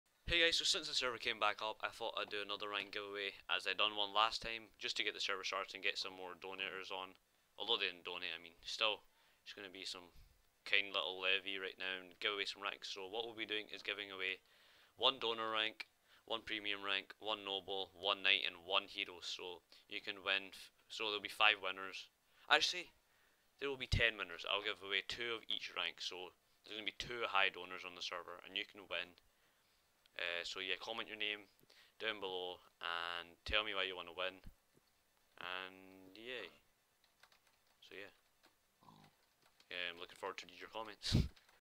FABLE PVP GIVEAWAY
WELCOME BACK TO FABLEPVP, TO START US OFF IM DOING A GIVEAWAY ALL YOU HAVE TO DO IS LEAVE A LIKE, COMMENT & LEAVE YOUR IGN AND WHY YOU WANT THE RANK

Giving away:
2 Donor ranks
2 Premium ranks
2 Noble ranks
2 Knight ranks
2 Hero ranks